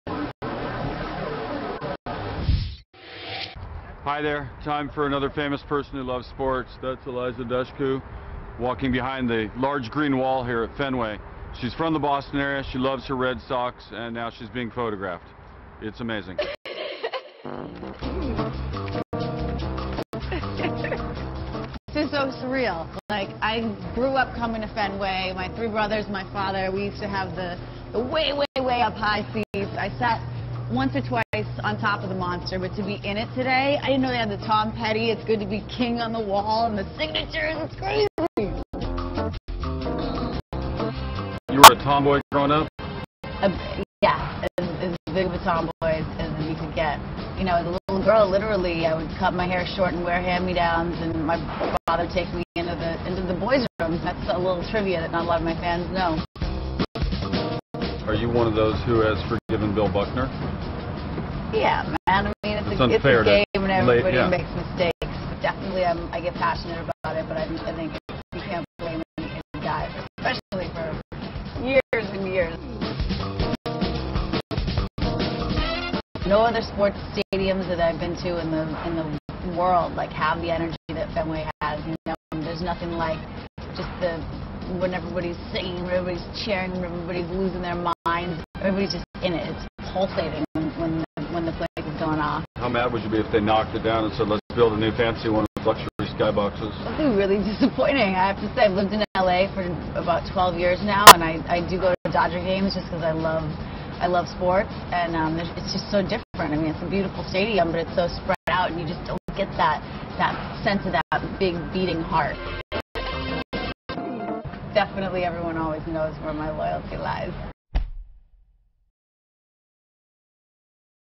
Fans Like Us: Eliza Dushku
Eliza Dushku talks with Kenny Mayne about the Red Sox